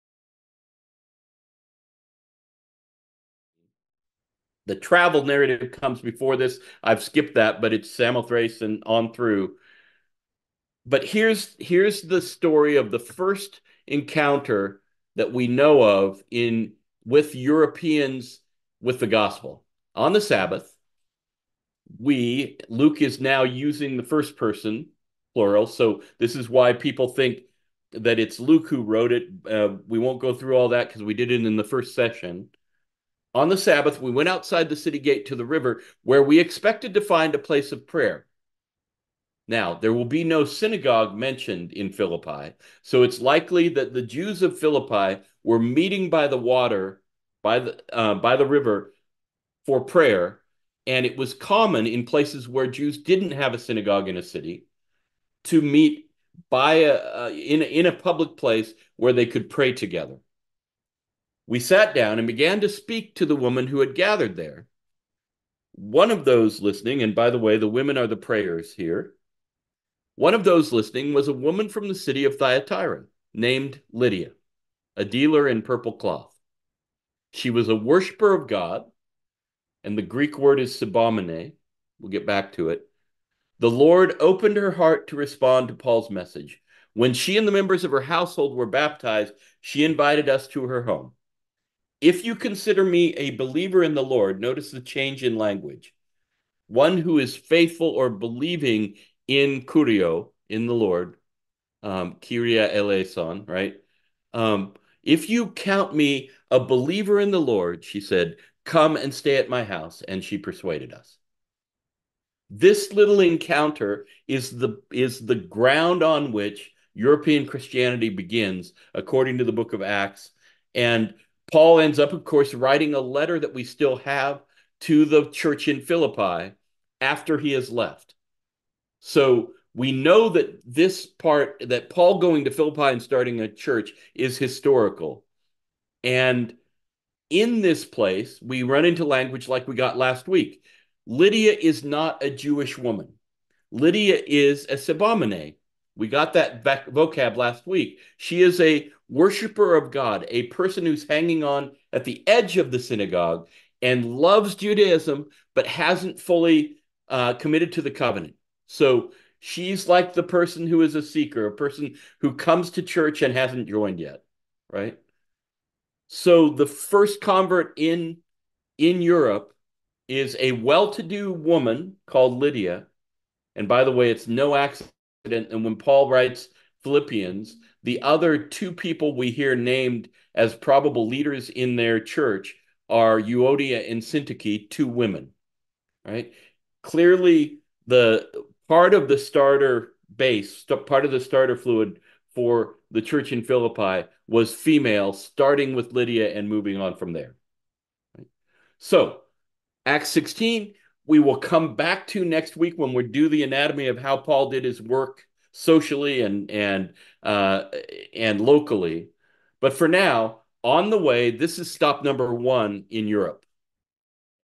The Sequel: Jesus Continued in the Book of Acts - Philippi
The Sequel: Jesus Continued in the Book of Acts with Rev. Dr. Allen Hilton, February 15, 2024. This clip is about Philippi.